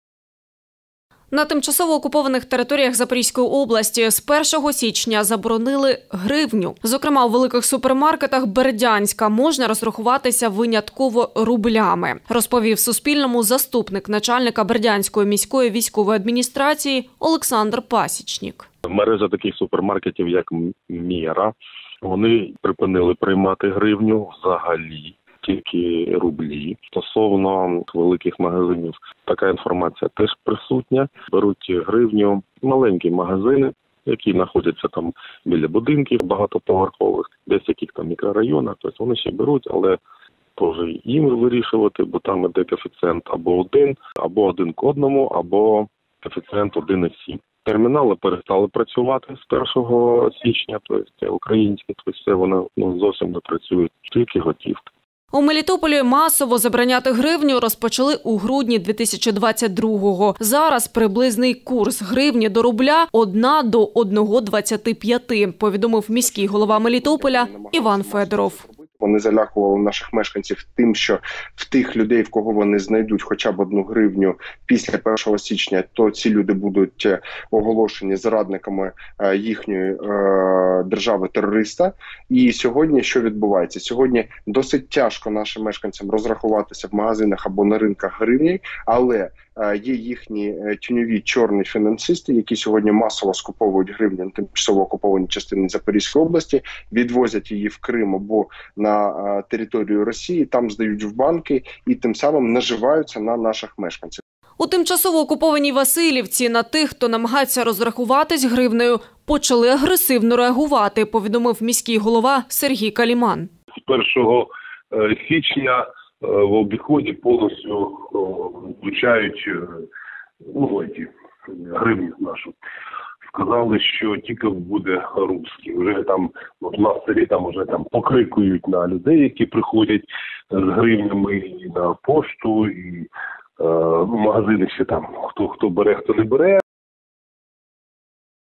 У тимчасово захоплених районах Запорізької області окупанти заборонили гривню | Новини
На тимчасово окупованих територіях Запорізької області з 1 січня заборонили гривню. Зокрема, у великих супермаркетах Бердянська можна розрахуватися винятково рублями. Про це розповів Суспільному заступник начальника Бердянської міської військової адміністрації Олександр Пасічник.